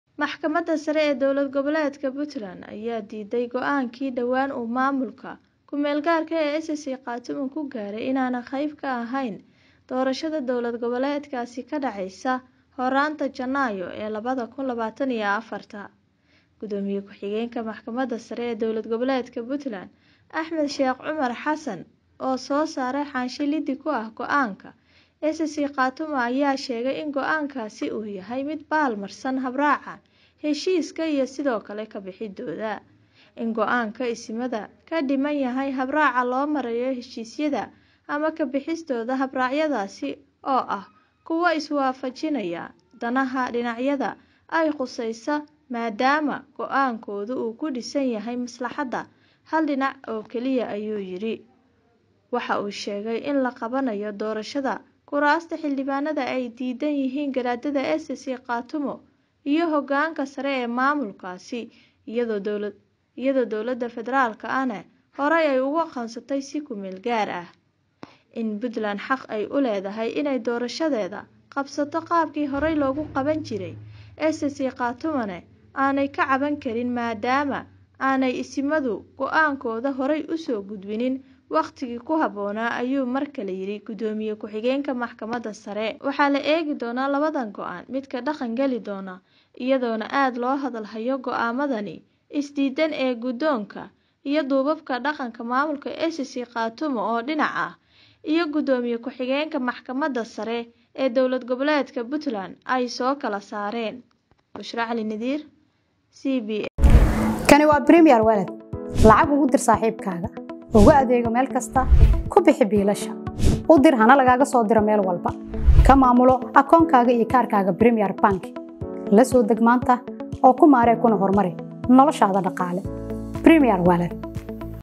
Guddoomiye ku xigeenka Maxkamadda Sare ee Puntland oo ka horyimid go’aanka Garaadada SSC-Khaatumo.
Guddoomiye ku xigeenka Maxkamadda Sare ee  Puntland oo ka horyimid go’aanka ay qaateen Garaadada SSC-Khaatumo.